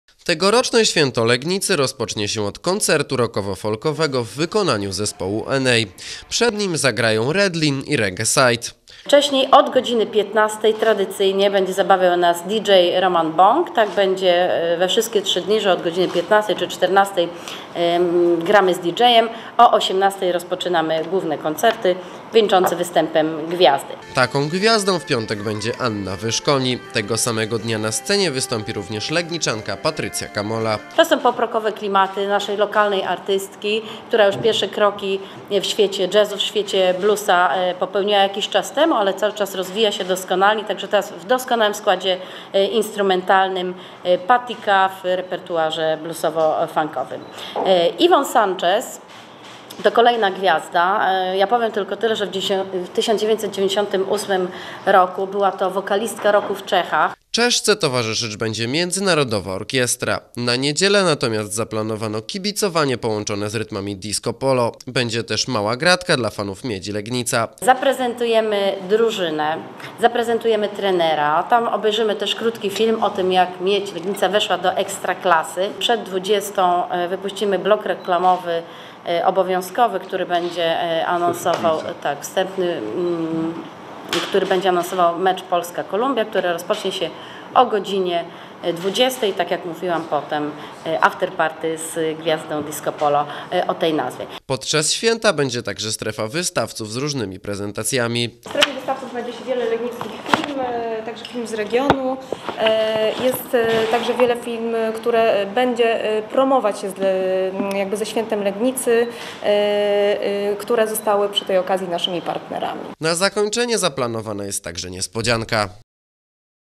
Atrakcje na Święcie Legnicy
W piątek, w sobotę i w niedzielę legniczanie mogą bawić się podczas święta miasta. Podczas trzech dni nie zabraknie dobrej muzyki, zabawy na wesołym miasteczku oraz piłkarskich emocji w czasie meczu Polaków na Mundialu.